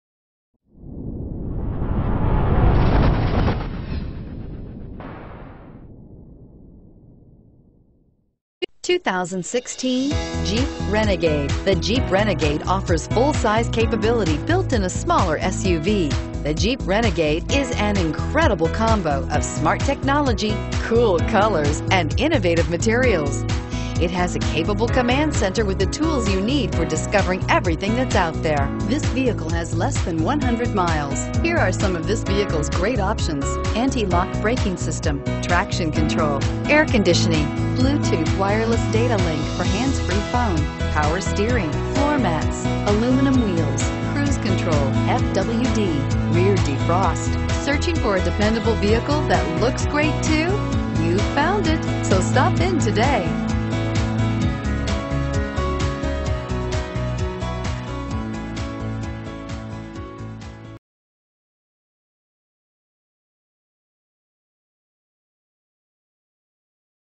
2016 Jeep Renegade Charlotte NC J63083
2016 Jeep Renegade Latitude

You'll love this 2016 Jeep Renegade. This is a suv you'll want to take home. With 3 miles, it features Automatic transmission and an exterior color of Glacier Metallic. Call and get in touch with Keffer Chrysler Jeep directly, and be the first to open the suv door today!